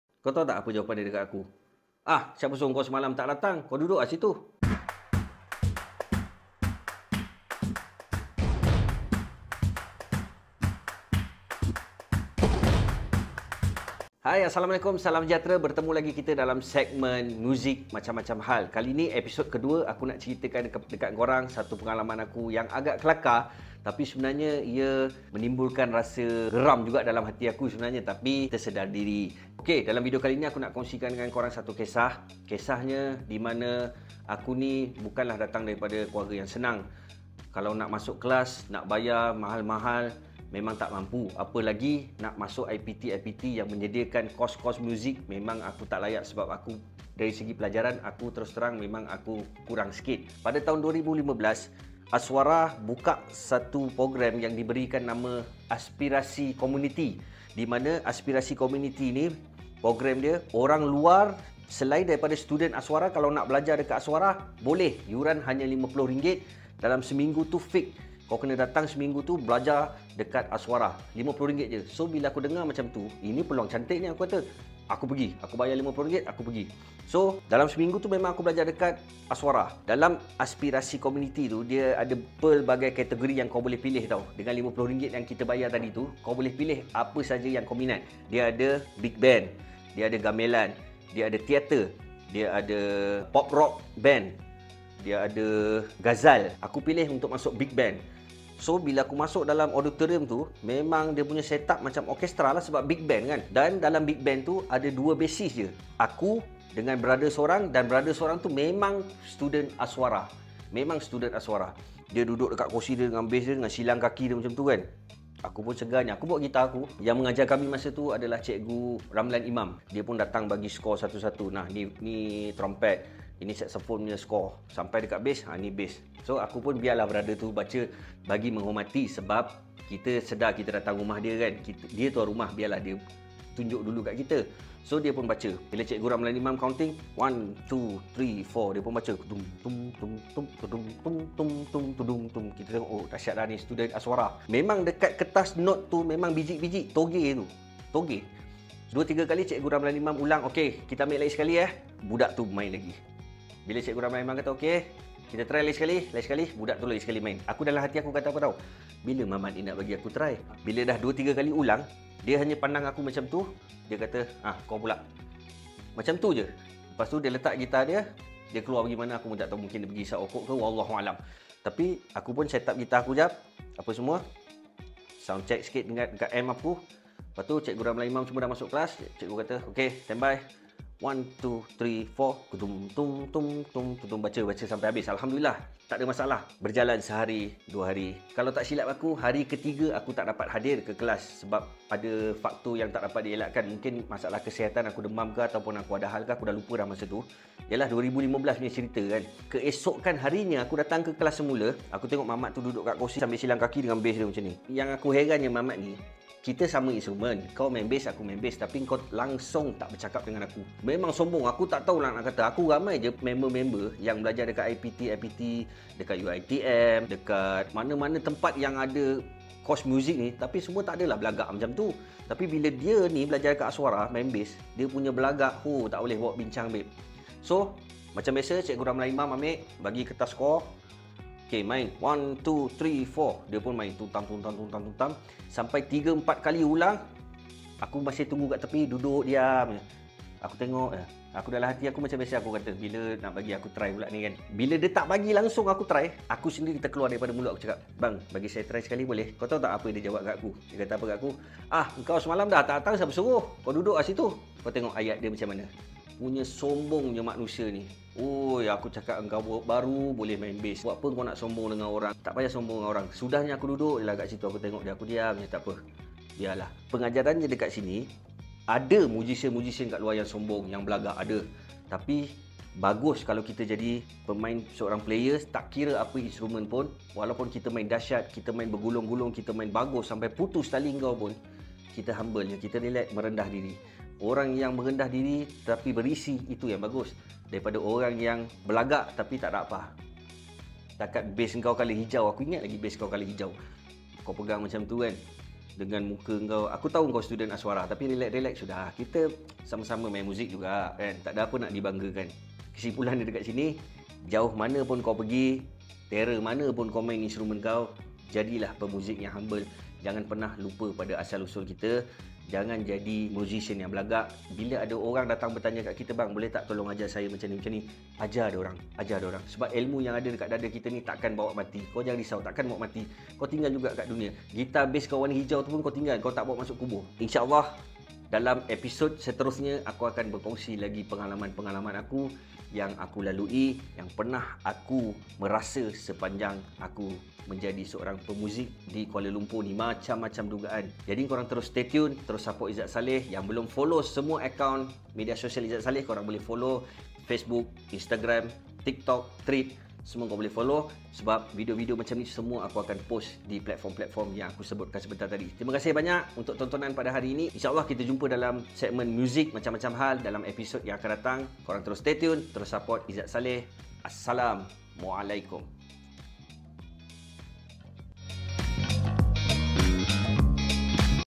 MUZIK..?? MACAM-MACAM HAL Episod 002
MUZIK..??
MACAM-MACAM HAL

EPISOD 002

Dalam episod kali ini..sy berkongsikan pengalaman sy pernah menyertai satu program di aswara…apa yg berlaku selepas itu..??

jom tonton…steady…🌹🌹